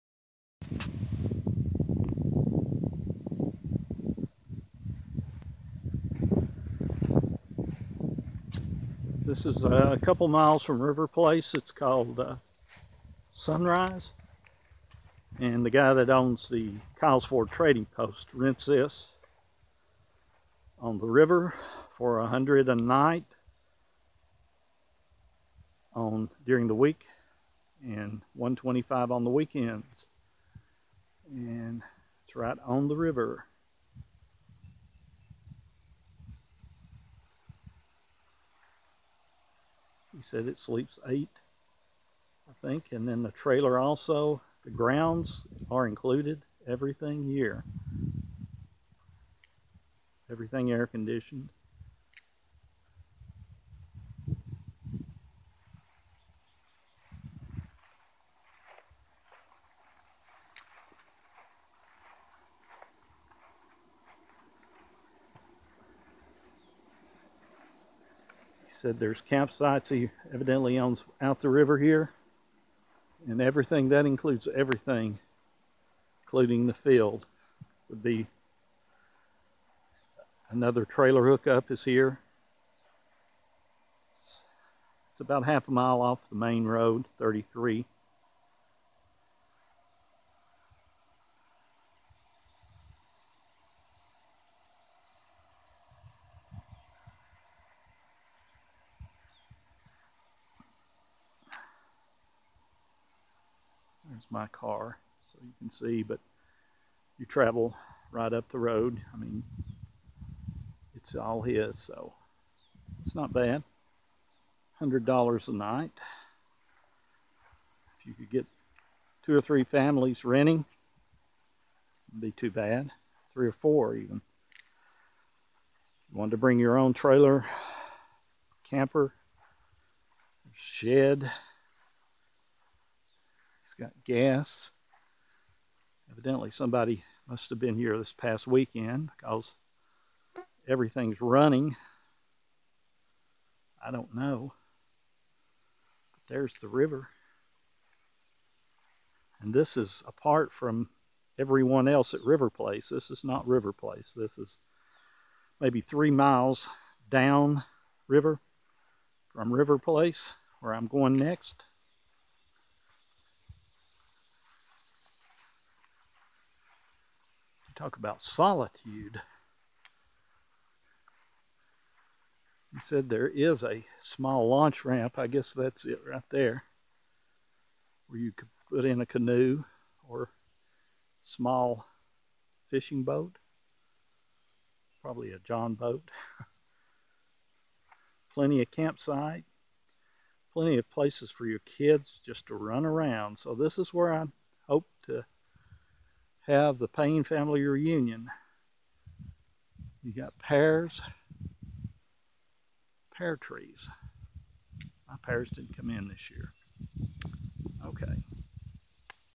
Sunrise Cabin Kyles Ford TN 3 miles from River Place on the Clinch 07-25-17
This new cabin and nice camper are rentals about 3 miles from River Place on the Clinch in Kyles Ford, TN.